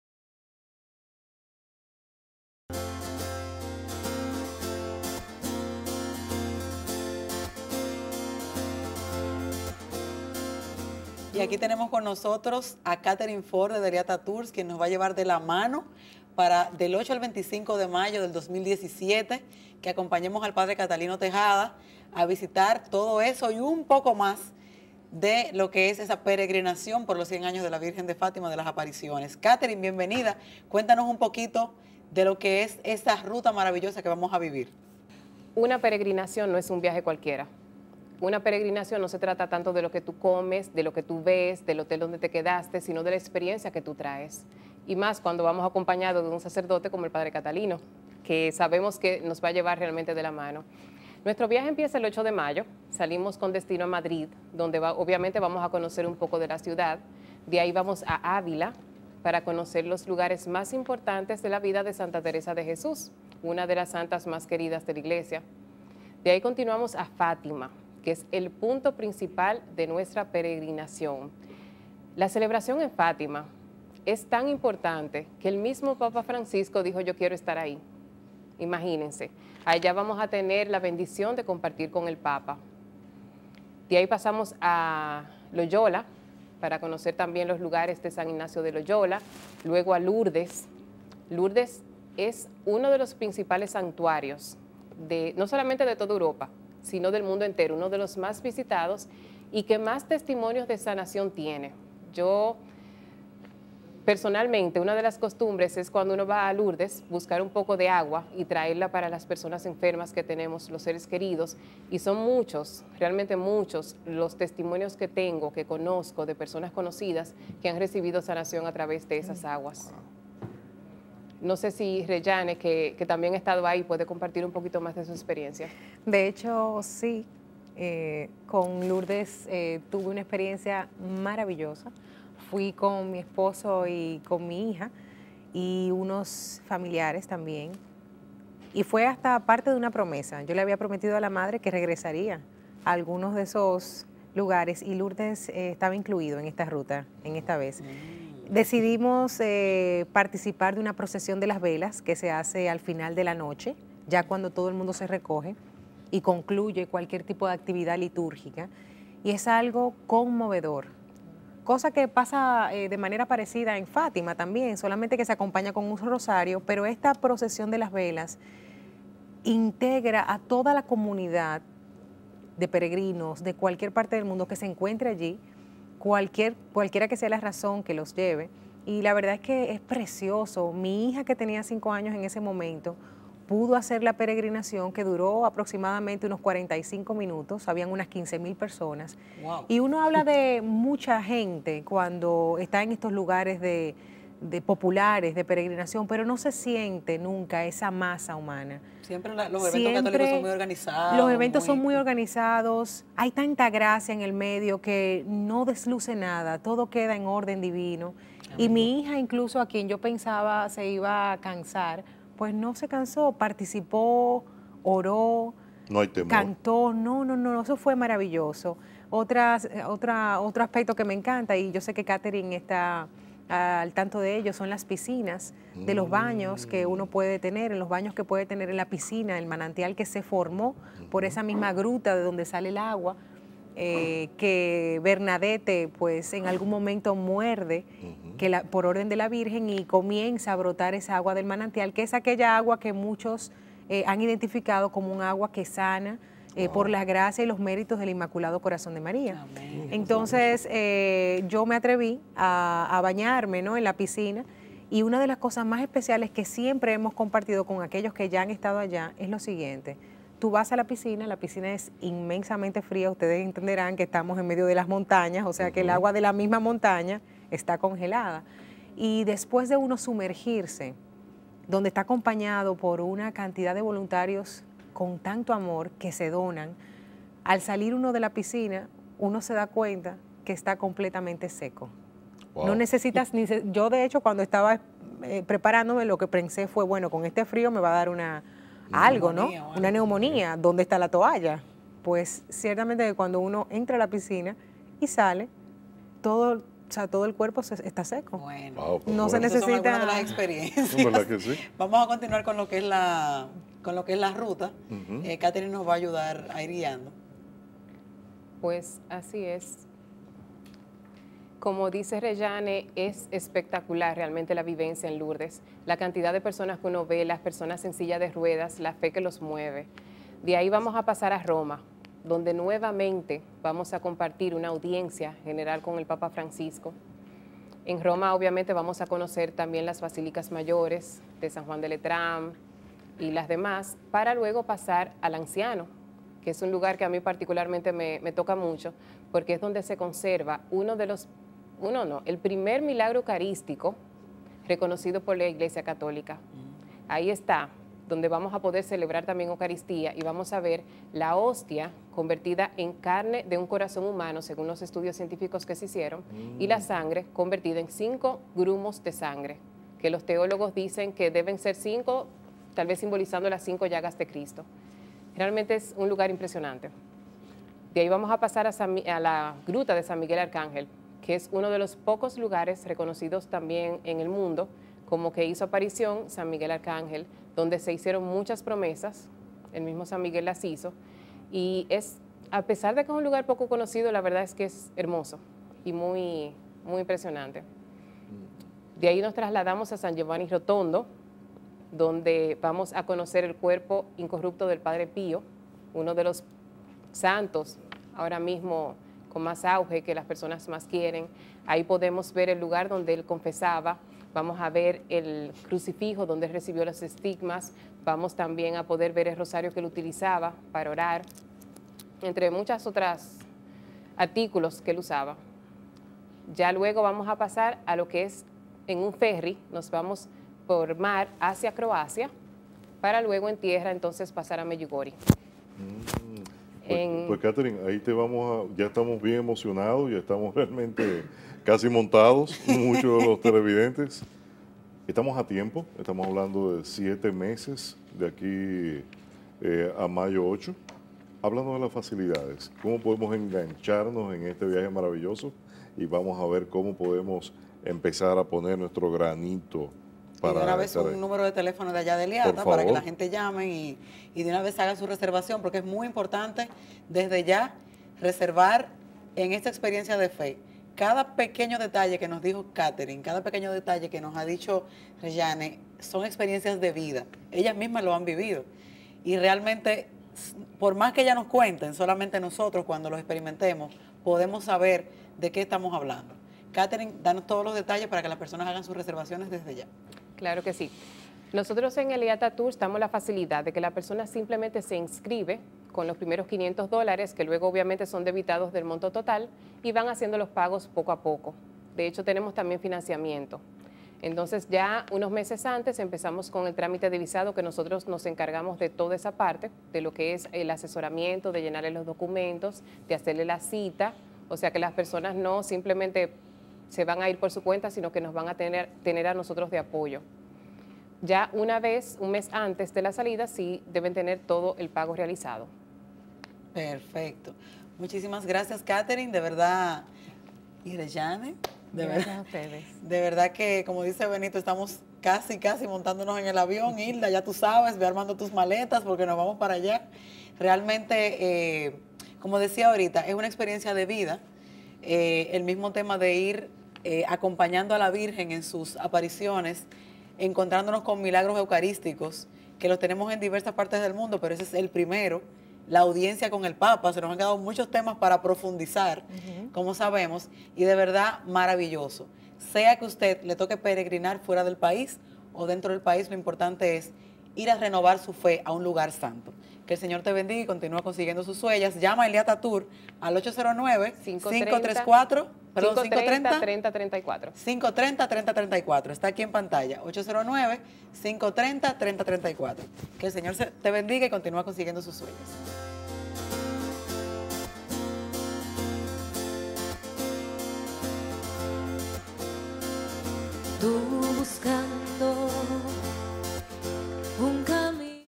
Noti-Capsula. Catherine Forde de Eliata Tours
Detalles de la peregrinación hacia Fatima para celebrar los 100 años de las apariciones de la Virgen.